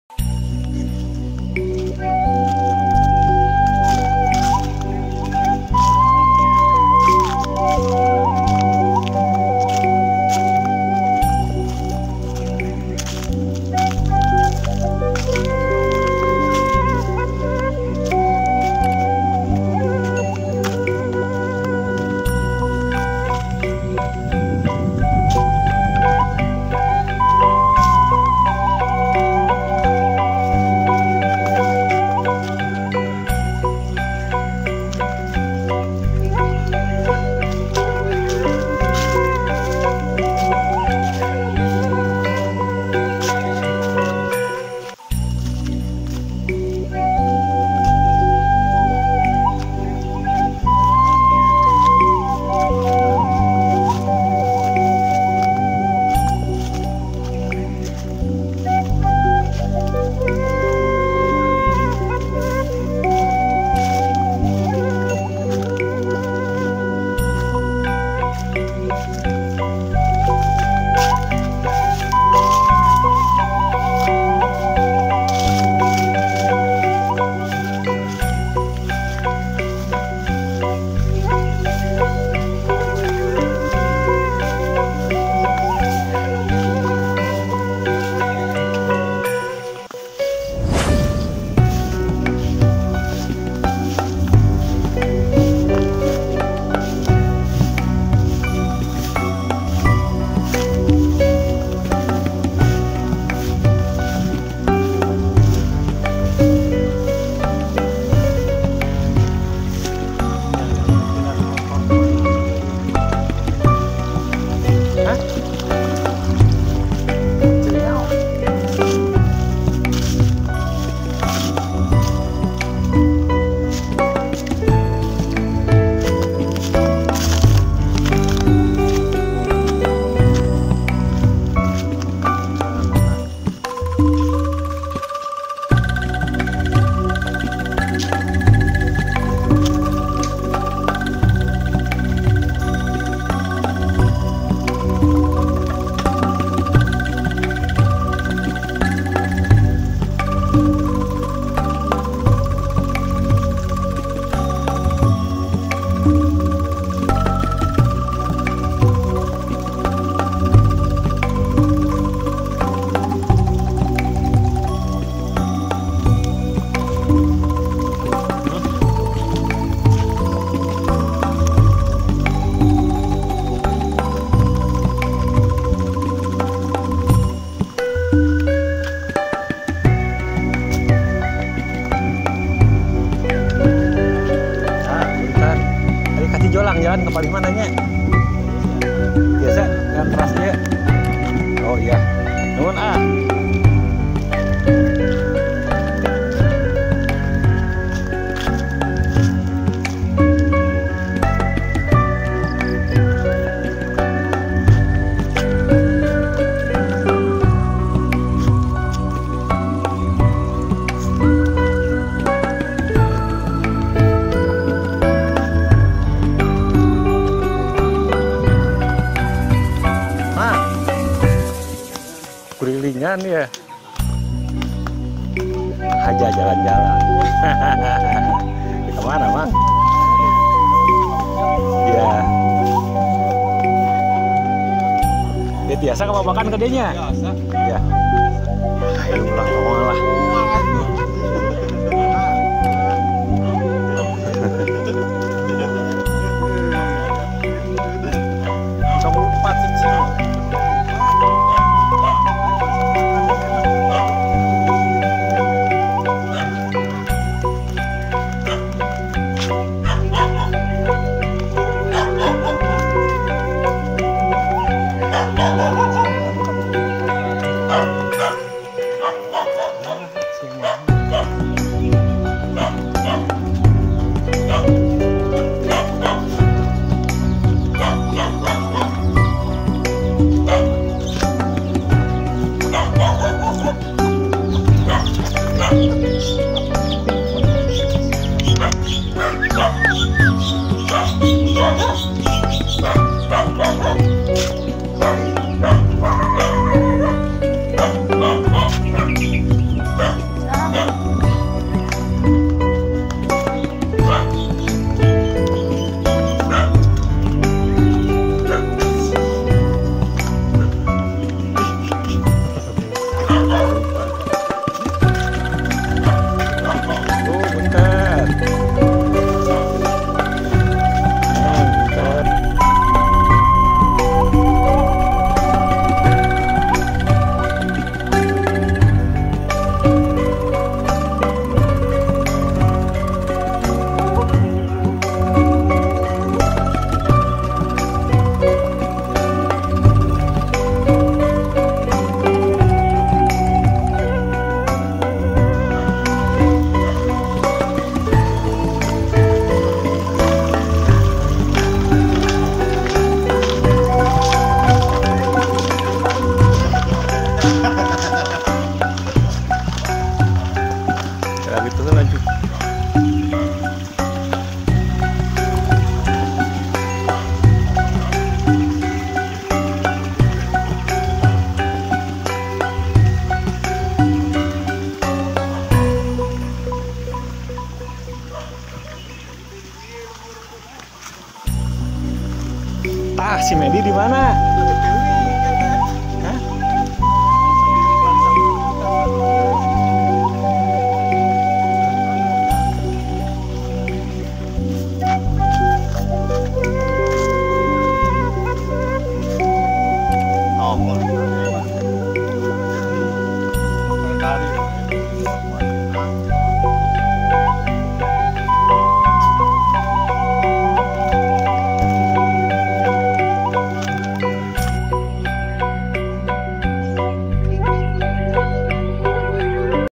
Hiking Ke Kampung Nanggewer Penampakan Mojang Desa Asli...!!! || Rindu Kampung
Hiking atau jalan kaki menyusuri kebun , sawah dan perkampungan sangat menyenangkan . Selain menyehatkan badan , juga tentunya menyegarkan mata kita...apalagi singgah di suatu perkampungan yang ada mojang nya...yang masih ORIGINAL khas gadis desa